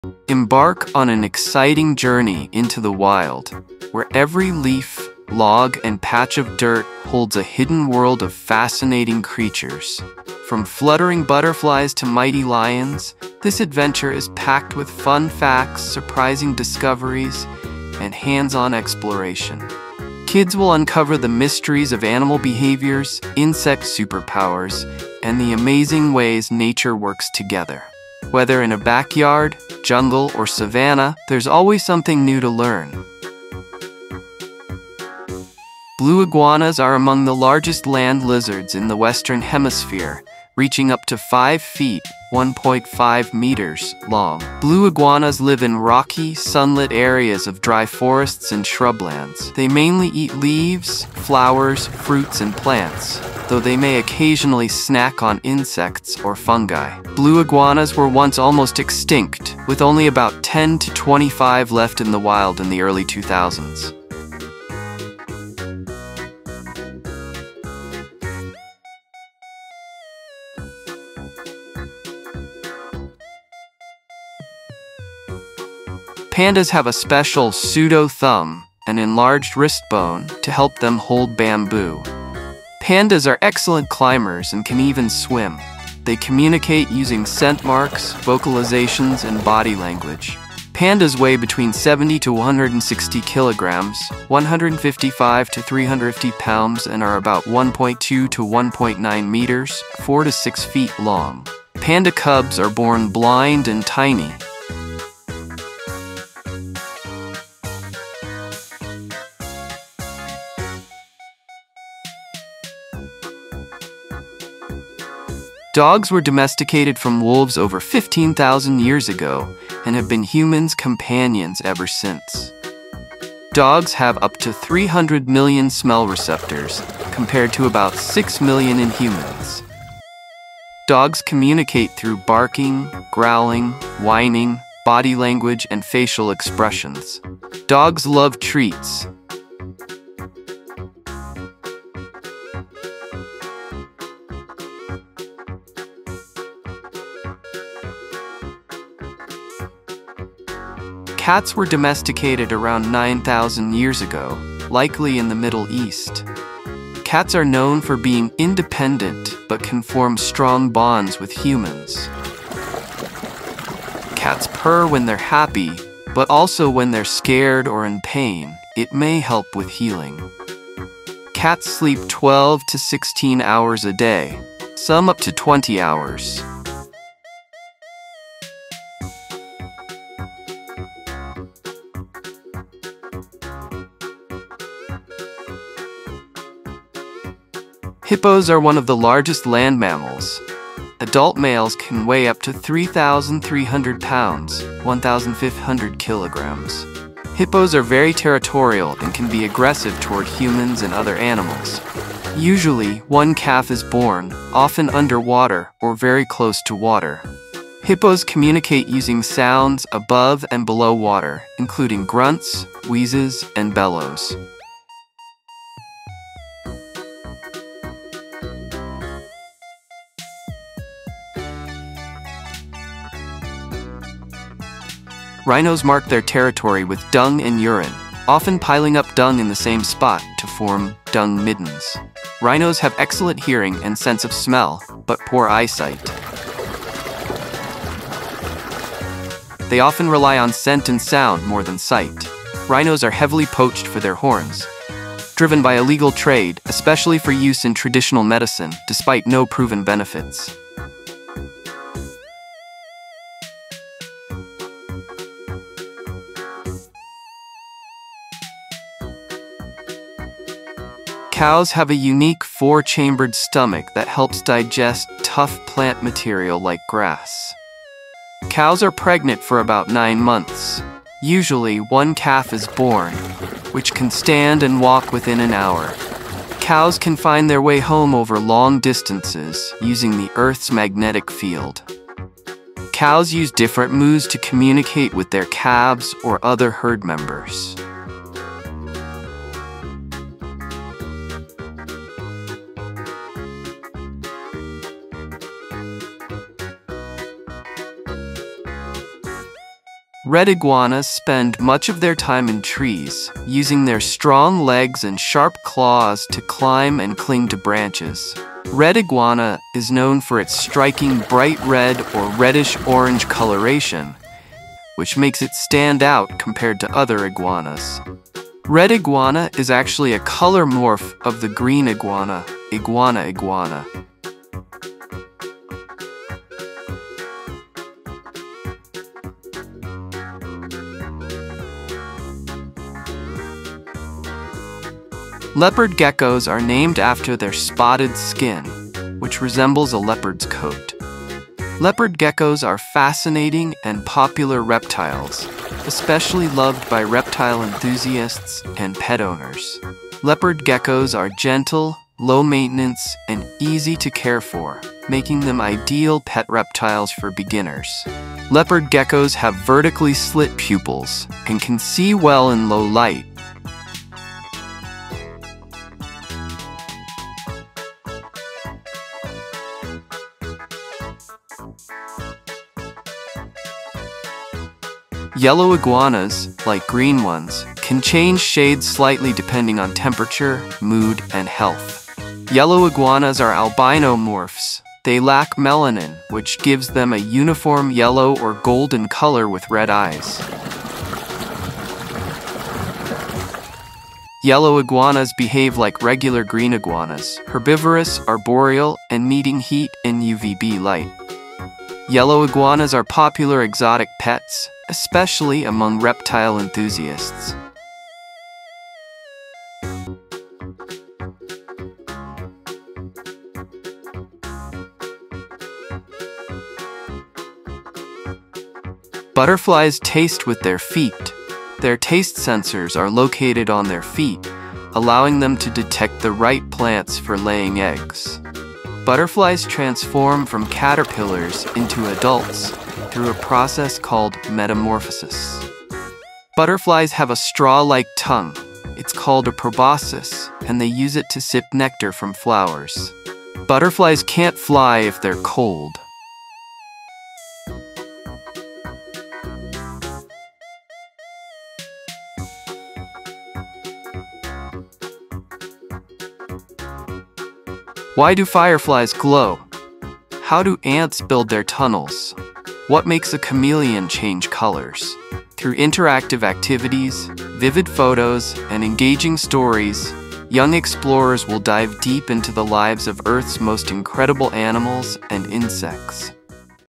A Kid’s Wild Expedition: Uncovering the Secrets of Animals and Insects!
Embark on an exciting journey into the wild, where every leaf, log, and patch of dirt holds a hidden world of fascinating creatures! From fluttering butterflies to mighty lions, this adventure is packed with fun facts, surprising discoveries, and hands-on exploration. Kids will uncover the mysteries of animal behaviors, insect superpowers, and the amazing ways nature works together. Whether in a backyard, jungle, or savanna, there’s always something new to learn—and this expedition is the perfect way to start!

Get ready to meet creepy crawlers, soaring birds, and stealthy predators—all while playing the role of a wildlife detective! Why do fireflies glow? How do ants build their tunnels? What makes a chameleon change colors? Through interactive activities, vivid photos, and engaging stories, young explorers will dive deep into the lives of Earth’s most incredible animals and insects. They’ll even learn how to spot clues in nature, like tracks, nests, and feeding signs, just like real scientists!

This wild adventure isn’t just about observing—it’s about inspiring curiosity, respect, and a love for the natural world. Kids will discover how every creature, big or small, plays a vital role in our planet’s story. With fun quizzes, DIY exploration challenges, and tips for safe wildlife watching, this expedition will turn every outdoor moment into a thrilling discovery. So grab a magnifying glass, put on your explorer’s hat, and let the wild wonders amaze you! 🌿🐞🦁

ToyTalkEnglish
toytalkenglish

Enjoy more enjoyable videos!